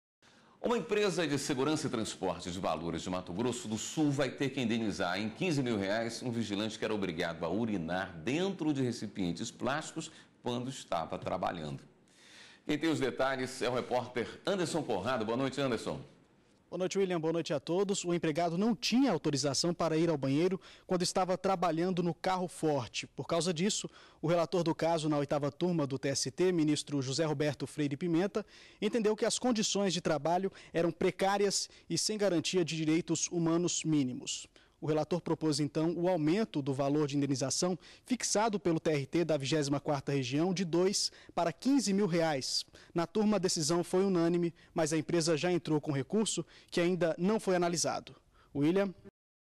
Condições precárias (11/07/17)
Empresa terá que indenizar em 15 mil reais, um vigilante que era obrigado a urinar dentro de garrafas plásticas no trabalho.